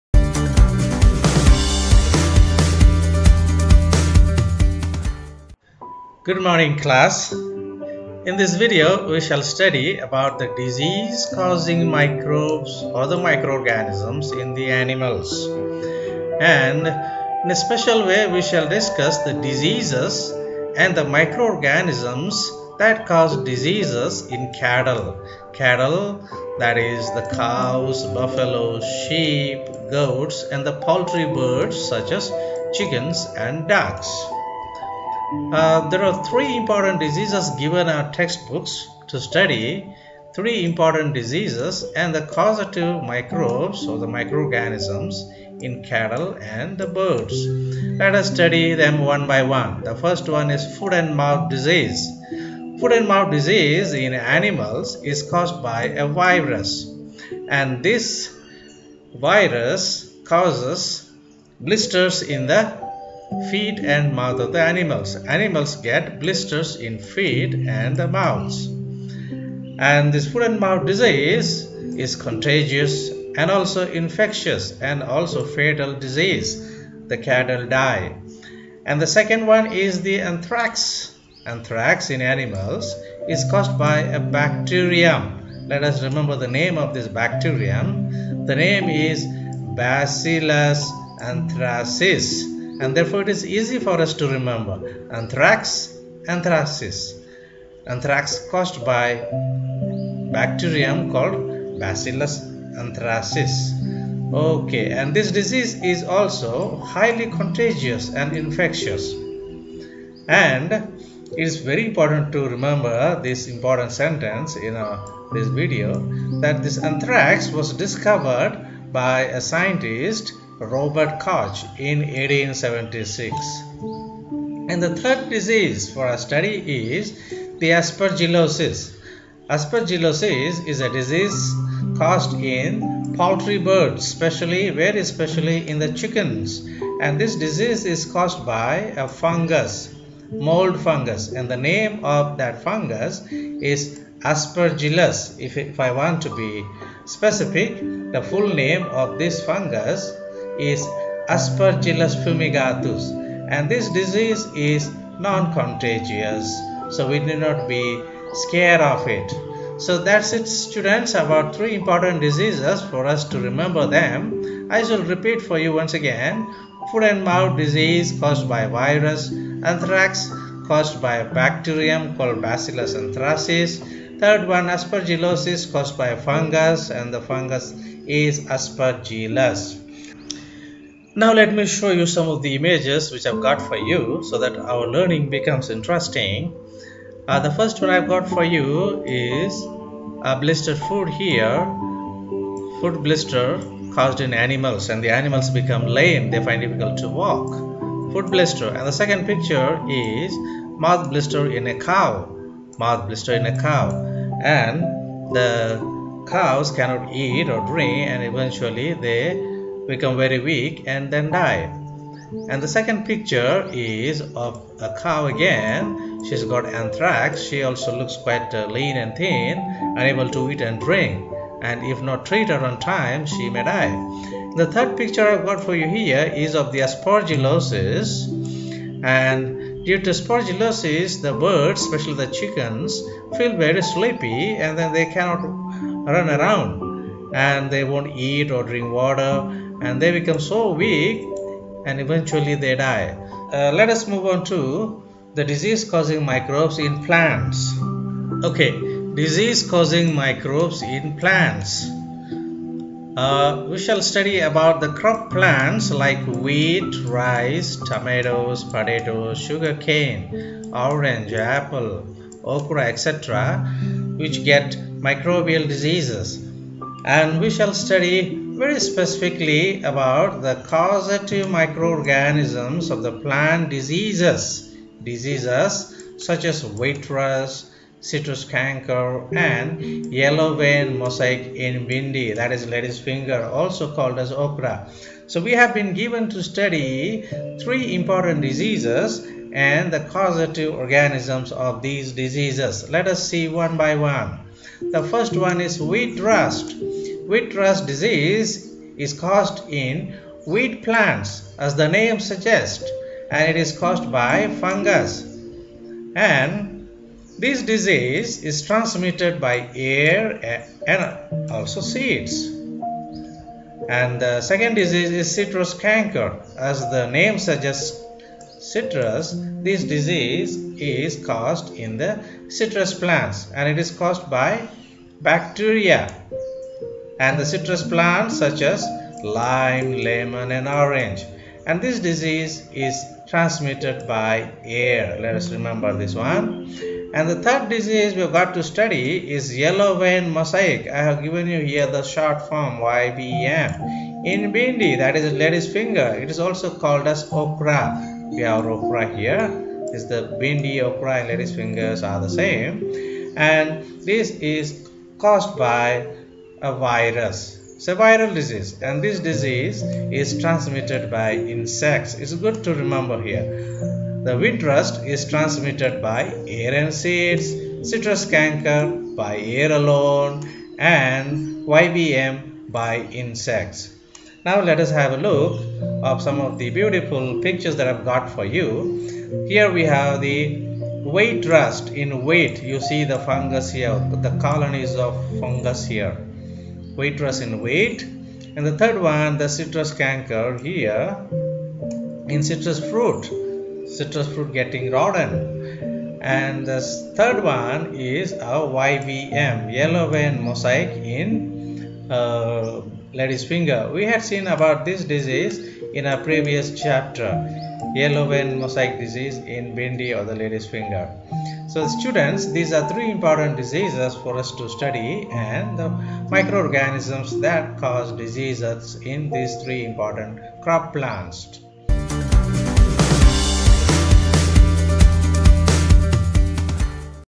Disease Causing Microorganisms in Animals & Plants - CBSE Class 8 Science
DISEASE-CAUSING MICROBES IN ANIMALS
Three diseases and causative microbes in cattle and birds –
Foot and Mouth disease – in animals - Virus
      Animals get blisters in feet and mouths.
      Contagious and Infectious and fatal disease.
Anthrax  - in animals - Bacterium – Bacillus anthracis
      Highly contagious and infectious disease
        Discovered by Robert Koch (1876).
Aspergillosis - Poultry birds – Fungus (mould) – Aspergillus
  Non-contagious disease